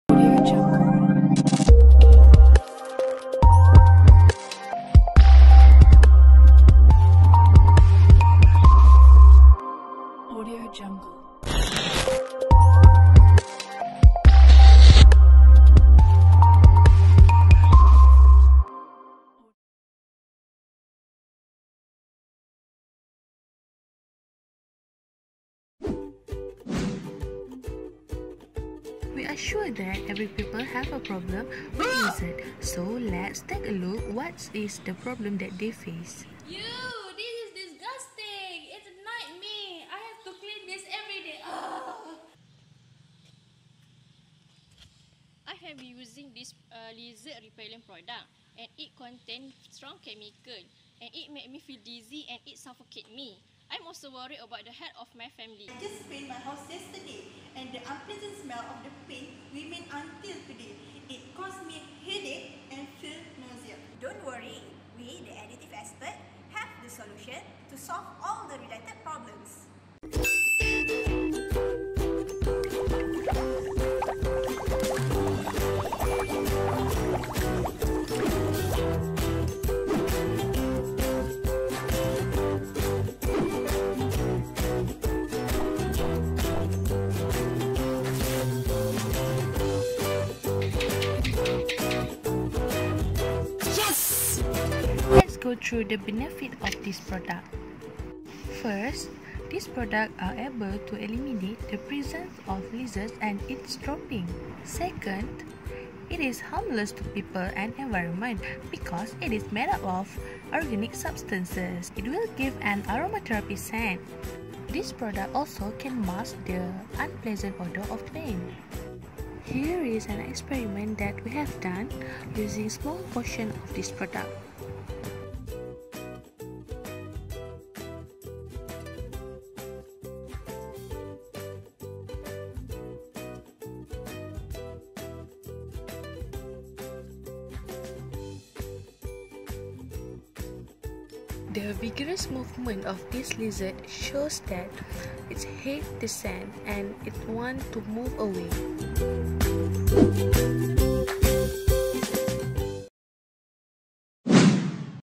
GDIID 2019 | ID 90 : NATURAL BASED ANTI-LIZARD ADDITIVE
Lizard or Hemidactylus frenatus is a native of Southeast Asia, which is very useful in keeping the house free from other insect or pest such as ants, flies, spiders, grasshoppers and potato bugs. However, its droppings can cause nuisance to human as the droppings tend to gather in corners, behind picture frame or can occasionally dribble down on the wall of house, mosque, hostel, hotel and others. The lizard’s dropping is toxic and unsanitary in nature as it consists of dangerous bacteria called Salmonella which can pose threat to human life when accidentally ingested the lizard’s dropping or the food items that get contaminated with the lizard. Our project reinvented the current existing product by developing new product “Natural based Anti-Lizard Additive”, whereby the additive solution contains a unique blend of natural ingredients compared to existing product brand that are made up of chemical substances which are able to eliminate the presence of lizards and its dropping. We also improved the existence product by adding natural fragrance oil that act as aromatherapy as well as able to mask the unpleasant odor of the paint. The product is packed into an amber glass bottle that is durable and able to ensure the product integrity for long term storage as it can protect the solution from ultraviolet rays. The product also has a child-resistance packaging which is equips with child locked cap that acts as safety cap on the bottle to reduce the risk of children ingesting the solution. By using the organic substances as raw materials, “Natural based Anti-Lizard Additive”is an environment friendly and it is an easy to use product as the consumer only needs to add and mix the additive into the paint or it can also be directly used as a spray.